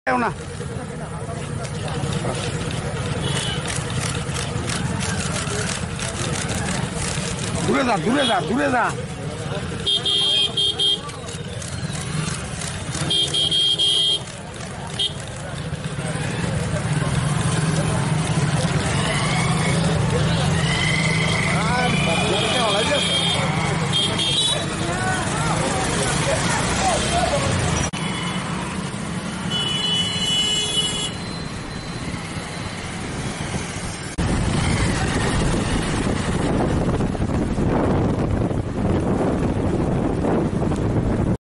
Ex-CM Manik Sarkar traveled across Agartala city on Strike day on two wheeler without Convoy.
Ex-CM Manik Sarkar traveled across Agartala city on Strike day on two wheeler without Convoy. TIWN video Jan 8, 2020